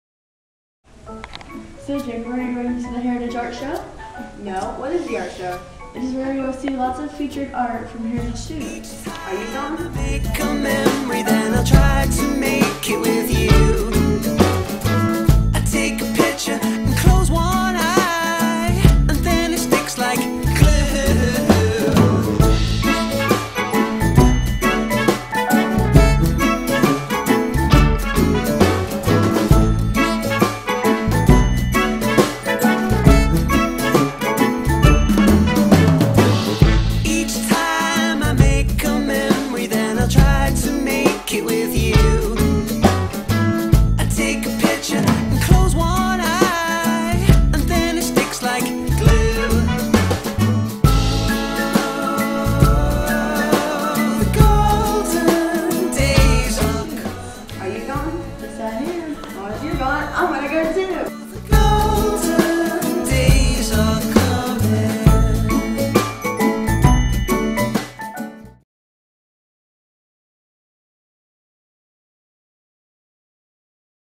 2012 Heritage Middle School Art Show Commercial
Miss Green and the Heritage Middle School Art Club proudly present the Heritage Middle School Art Show, Tuesday, March 6 at 5:30 P.M.